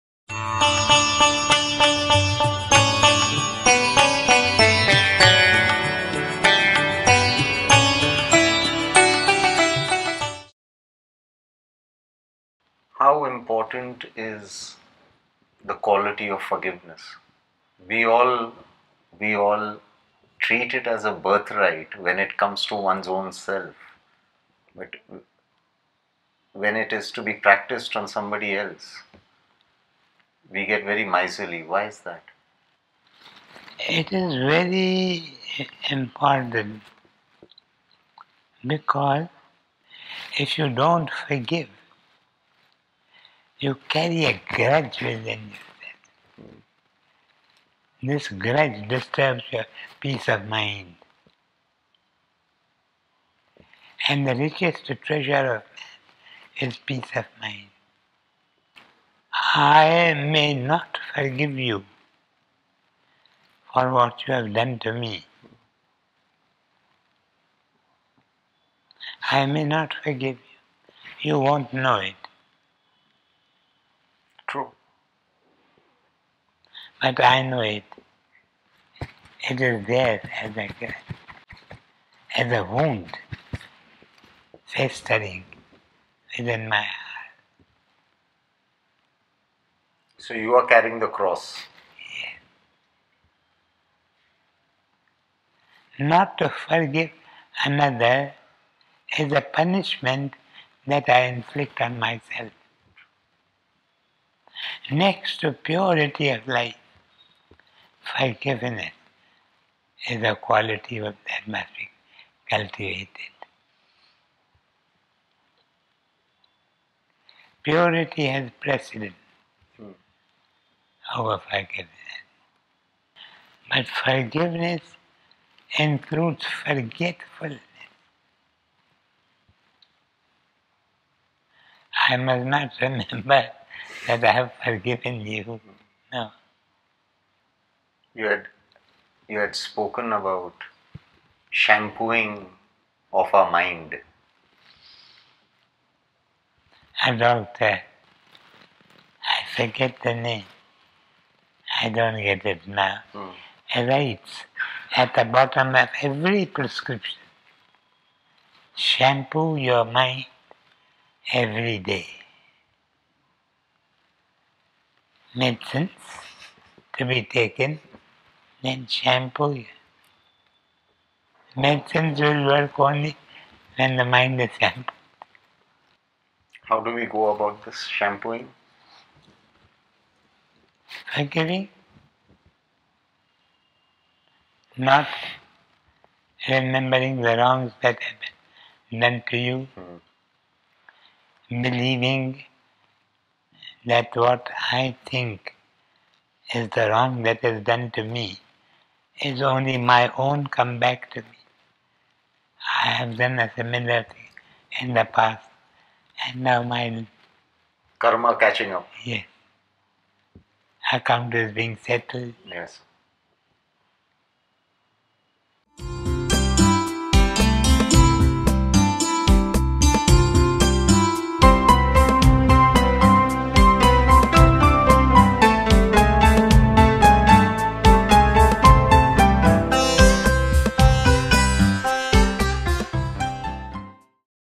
Dada and Ruzbeh : Forgiveness is a Choice
If you don’t forgive, you carry a grudge within you. This grudge disturbs your peace of mind, and the richest treasure of man is his peace of mind.